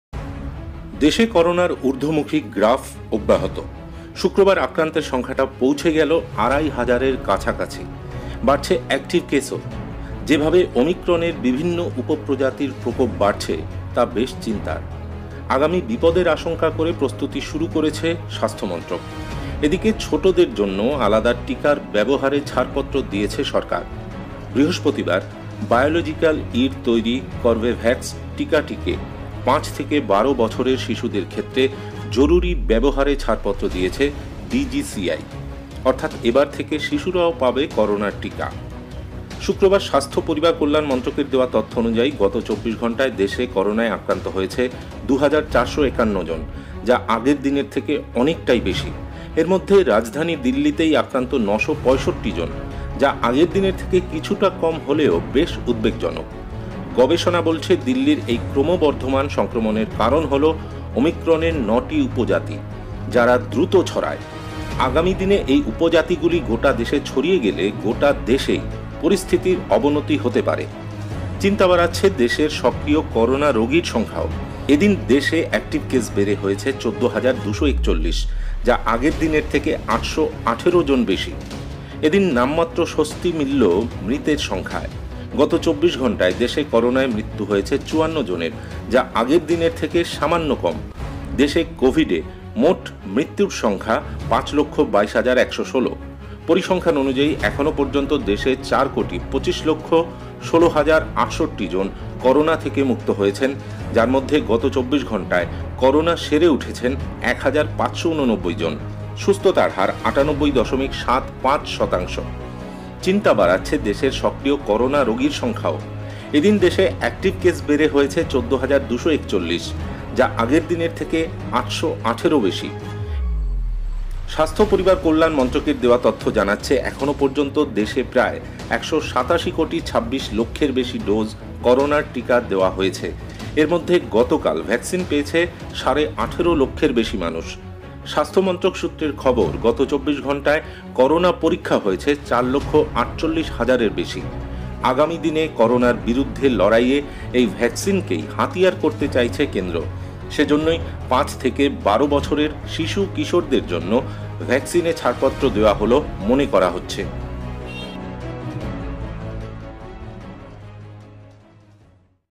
দেশে ফের ঊর্ধ্বমুখী সংক্রমণ, গত ২৪ ঘন্টায় দিল্লিতে আক্রান্ত প্রায় হাজার |Oneindia Bengali
দেশে ফের ঊর্ধ্বমুখী সংক্রমণ, গত ২৪ ঘন্টায় দিল্লিতে আক্রান্ত প্রায় হাজার